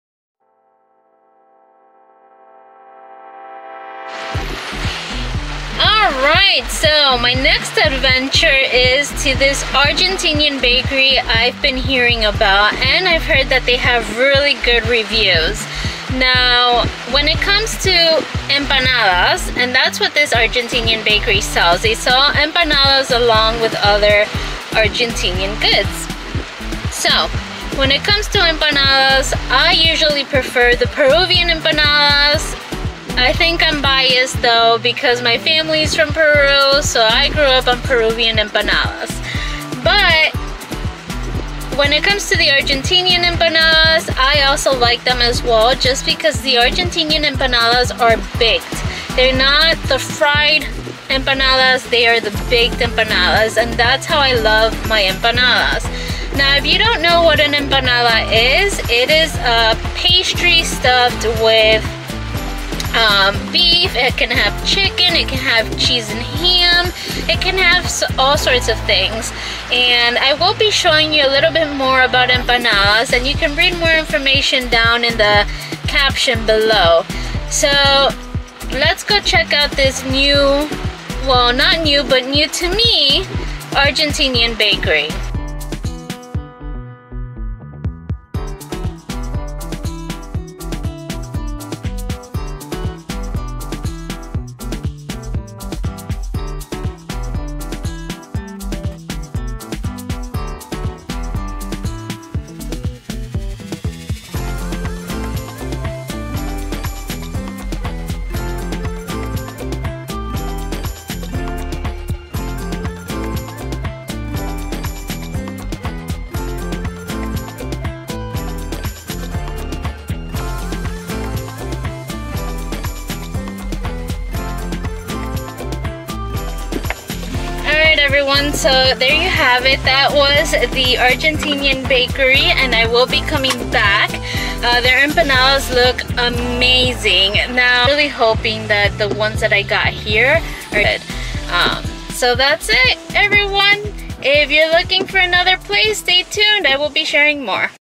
Trying Argentinian Empanadas in Orlando!
Come with me as I visit an Argentinian Bakery & Cafe in Orlando! While we have several places where you can find some good empanadas, this one is quite a gem with raving reviews. Plus, there is quite a selection of empanadas and an awesome Messi wall.

Links (equipment I use):

Ulanzi RMT-01
Sony ZV-E10 Bundle
Sony ZV-E10

TimeStamps:
0 - 1:24: Intro
1:25: Argentinian Bakery & Cafe
2:06: End